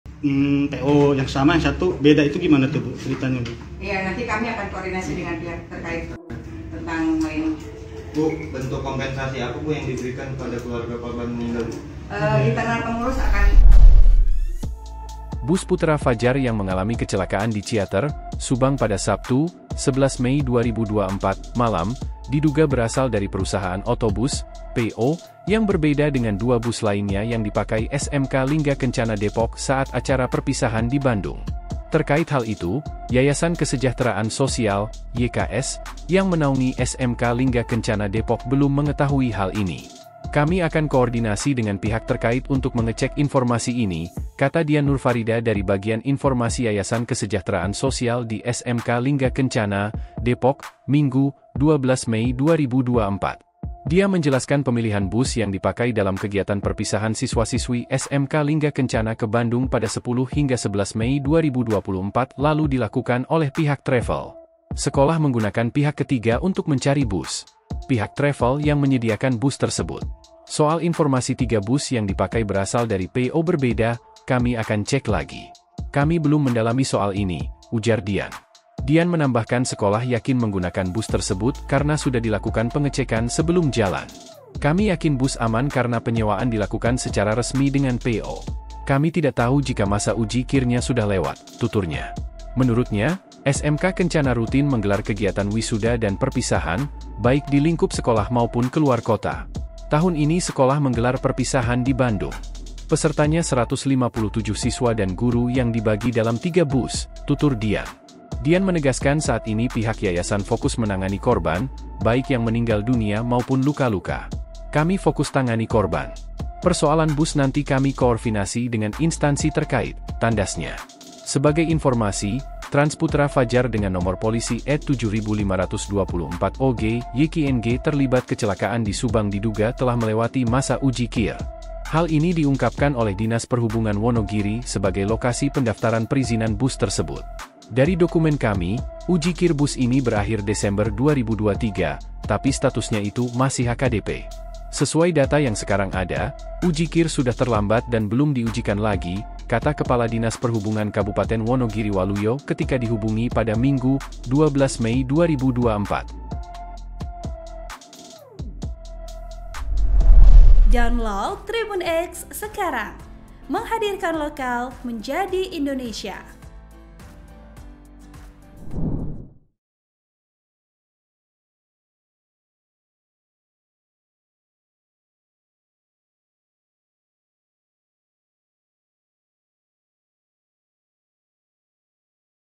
Bus Kecelakaan Maut di Subang Diduga dari PO Berbeda dari 2 Bus Lainnya
Bus Putera Fajar yang mengalami kecelakaan di Ciater, Subang pada Sabtu (11/5/2024) malam, diduga berasal dari perusahaan otobus (PO) yang berbeda dengan dua bus lainnya yang dipakai SMK Lingga Kencana Depok saat acara perpisahan di Bandung.

Terkait hal itu, Yayasan Kesejahteraan Sosial (YKS) yang menaungi SMK Lingga Kencana Depok belum mengetahui hal ini.

"Kami akan koordinasi dengan pihak terkait untuk mengecek informasi ini," kata Dian Nurfarida dari Bagian Informasi Yayasan Kesejahteraan Sosial di SMK Lingga Kencana, Depok, Minggu (12/5/2024).

Dia menjelaskan pemilihan bus yang dipakai dalam kegiatan perpisahan siswa-siswi SMK Lingga Kencana ke Bandung pada 10-11 Mei 2024 lalu dilakukan oleh pihak travel.

"Sekolah menggunakan pihak ketiga untuk mencari bus. Pihak travel yang menyediakan bus tersebut. Soal informasi tiga bus yang dipakai berasal dari PO berbeda, kami akan cek lagi. Kami belum mendalami soal ini," ujar Dian.

Dian menambahkan sekolah yakin menggunakan bus tersebut karena sudah dilakukan pengecekan sebelum jalan.

"Kami yakin bus aman karena penyewaan dilakukan secara resmi dengan PO. Kami tidak tahu jika masa uji KIR-nya sudah lewat," tuturnya.

Menurutnya, SMK Kencana rutin menggelar kegiatan wisuda dan perpisahan, baik di lingkup sekolah maupun keluar kota.

"Tahun ini sekolah menggelar perpisahan di Bandung. Pesertanya 157 siswa dan guru yang dibagi dalam 3 bus," tutur Dian.

Dian menegaskan saat ini pihak yayasan fokus menangani korban, baik yang meninggal dunia maupun luka-luka.

"Kami fokus tangani korban. Persoalan bus nanti kami koorfinasi dengan instansi terkait," tandasnya.

Sebagai informasi, Trans Putera Fajar dengan nomor polisi AD 7524 OG yqng terlibat kecelakaan di Subang diduga telah melewati masa uji KIR.

Hal ini diungkapkan oleh Dinas Perhubungan Wonogiri sebagai lokasi pendaftaran perizinan bus tersebut.

"Dari dokumen kami, uji KIR bus ini berakhir Desember 2023, tapi statusnya itu masih AKDP. Sesuai data yang sekarang ada, uji KIR sudah terlambat dan belum diujikan lagi," kata Kepala Dinas Perhubungan Kabupaten Wonogiri Waluyo ketika dihubungi pada Minggu (12/5/2024).